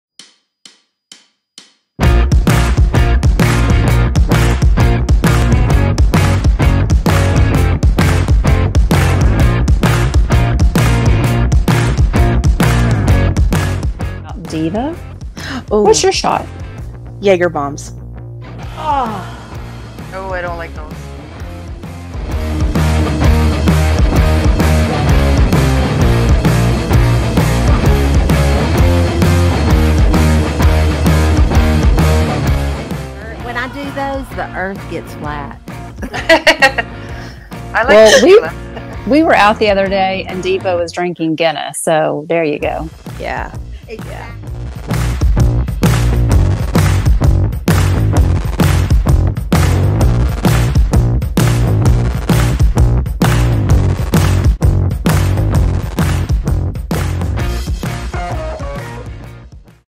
DIVA’s Jager Bomb
JAGER BOMB RECIPE:

1.5 oz Jagermeister IN A SHOT GLASS
1/2 can RedBull IN A PINT GLASS

MAKE A TOAST!
DROP THE JAGER SHOT IN THE REDBULL!
DOWN THE HATCH! 😁